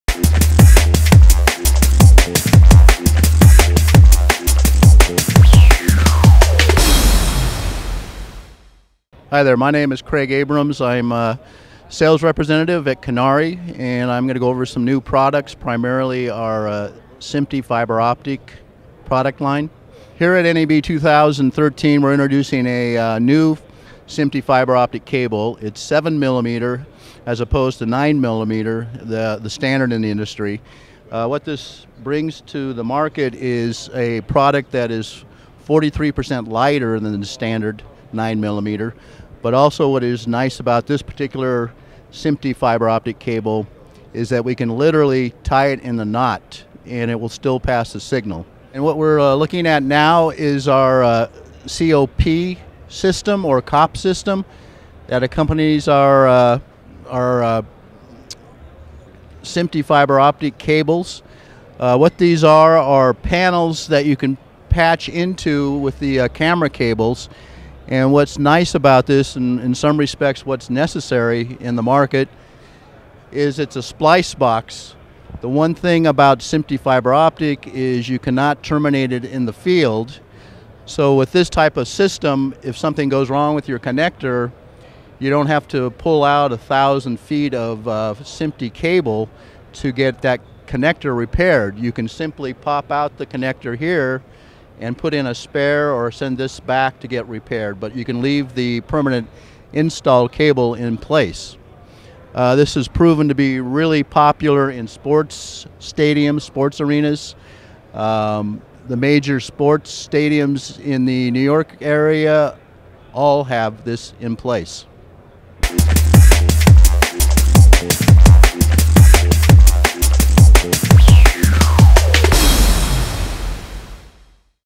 Canare Hybrid Fiber Optic Interfacing
Canare FCC Series 9.2mm Hybrid Fiber Optic SMPTE HDTV Cables

Canare Hybrid Fiber Optic Male to Female Cable assemblies are made with Canare LF-2SM9R 9.2mm Cable and are designed for studio and broadcast applications.

Use For:
HD-SDI
HDTV Camera Systems
Teleco Broadband
OB Vans
Sports Events

Specifications:
Meets SMPTE and ARIB Standard
Smooth and Flexible
Cable features Polyurethane jacket provides tear resistant characteristics
Elastomer used in the sheath construction ensures superb mechanical strength.